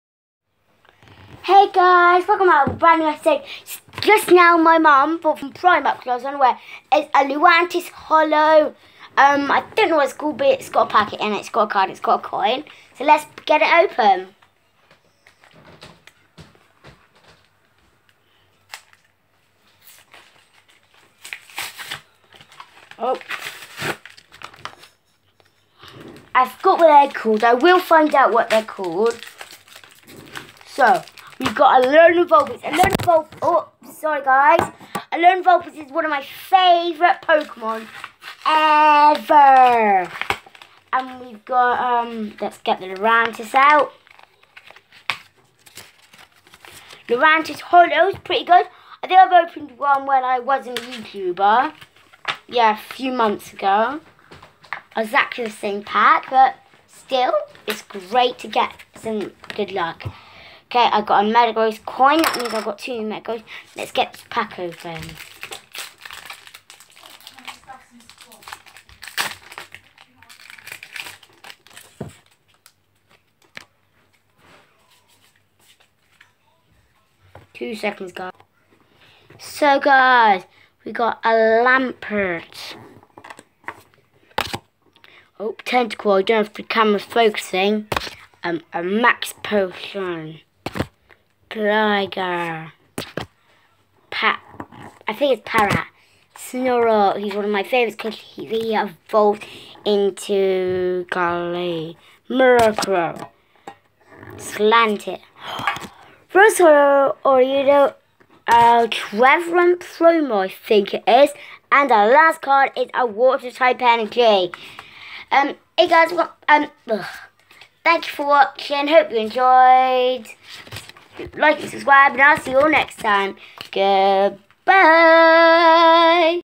Another card review.
The boy wants to open a whole box of Pokemon boosters for his next video, doesn't realise how much that will be.